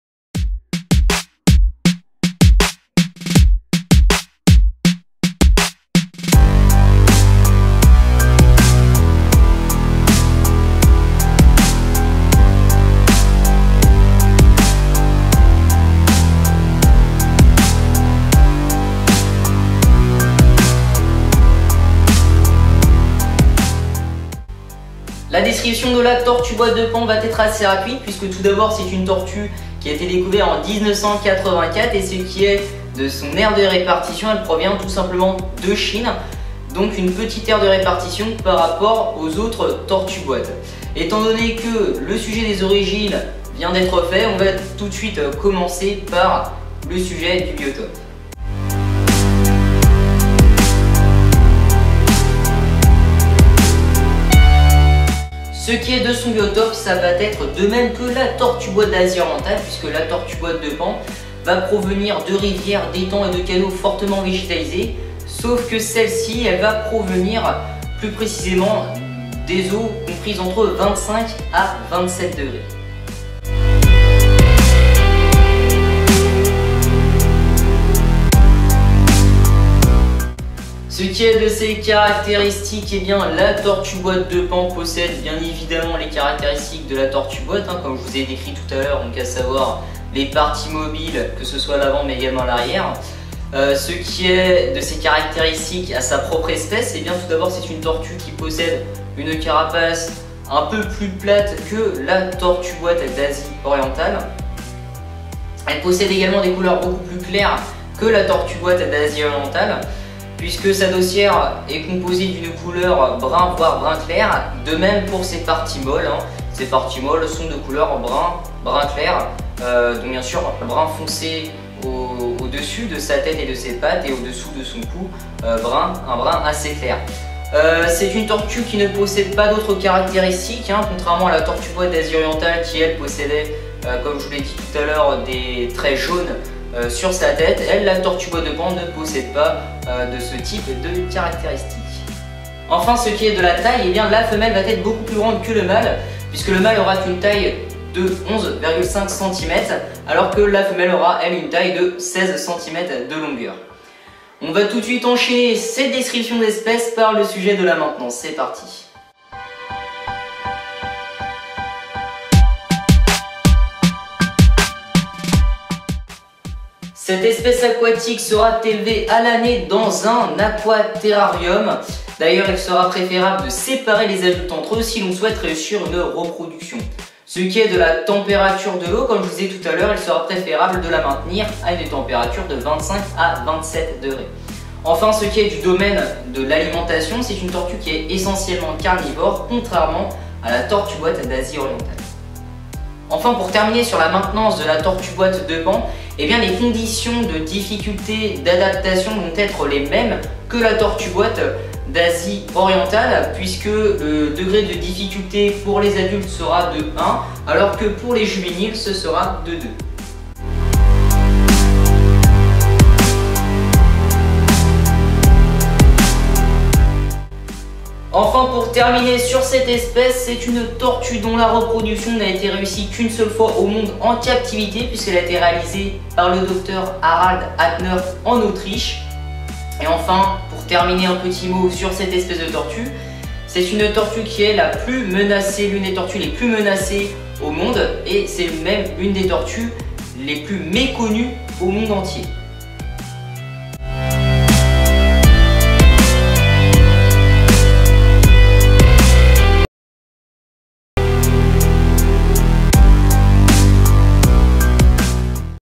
LA TORTUE-BOÎTE DE PAN - CUORA PANI.
L'étude sera portée sur la tortue-boîte de pan, "Cuora pani" une tortue qui appartient à la famille des sept espèces de tortues-boîtes. Elle possède également une caractéristique qui fait l'unicité de son espèce.